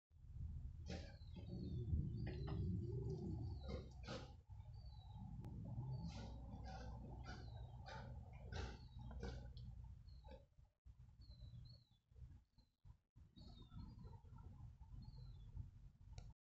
Rottweiler trotting in Garden Protection
Rottweiler trotting around the garden protecting his space with hundreds of toys which he doesn't break